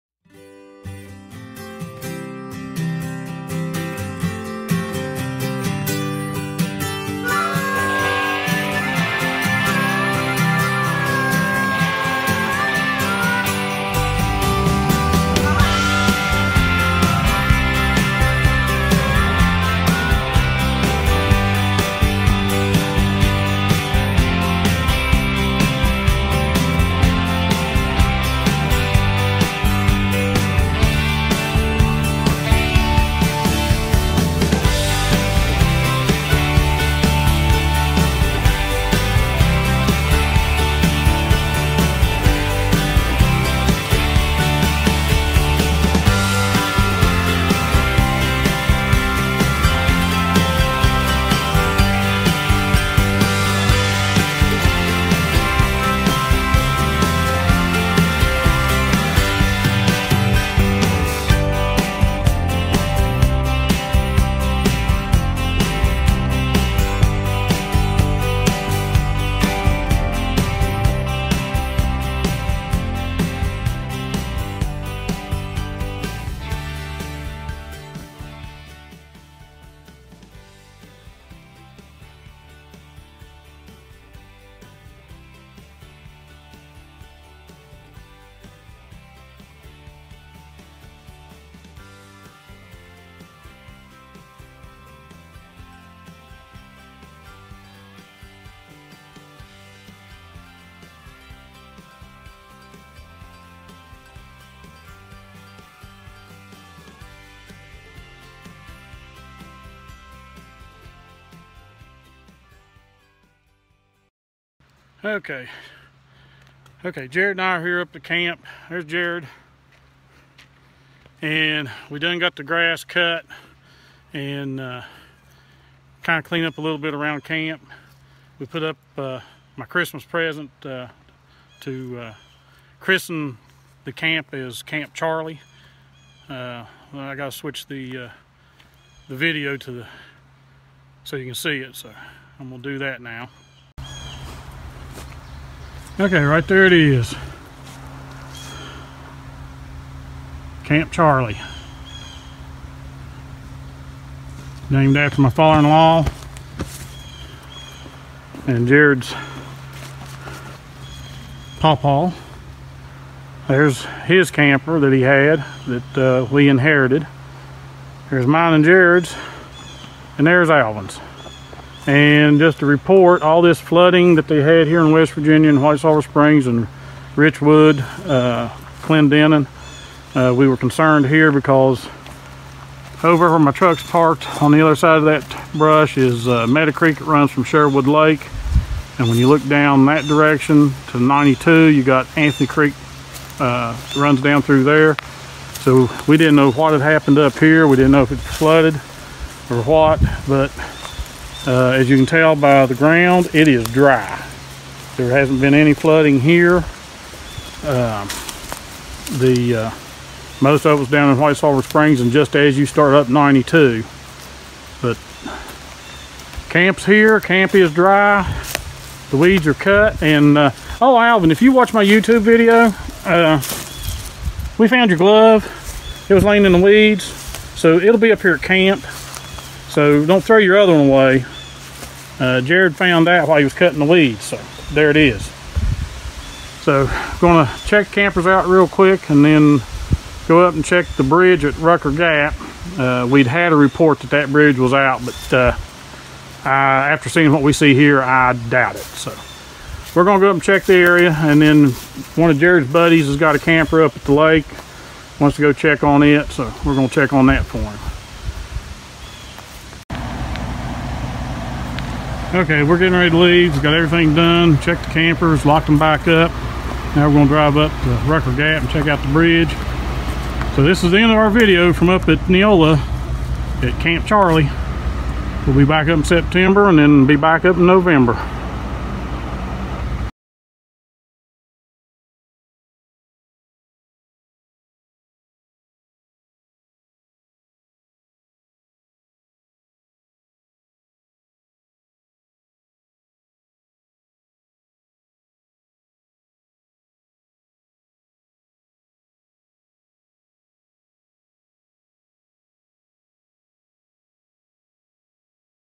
Checking Camp At Neola WV After Floods of June 2016
First we would like to say our prayers and hearts go out to the people of WV who were effected by the flood, and also to those who lost loved ones.  On June 23rd and 24th West Virginia was hit with a tremendous amount of rain.  They are saying it was a 1000 year storm, some areas saw close to 10 inches of rain.  Devastating floods destroyed several communities and towns, such as Clendenin, Rainelle, RIchwood and White Sulphur Springs.  White Sulphur Springs is south by only 16 miles from our camp site at Neola, WV.  Route 92 is the road we take from White Sulphur Springs to camp.  We made a visit to camp today, July 1st, to see if we had any damage to our campers.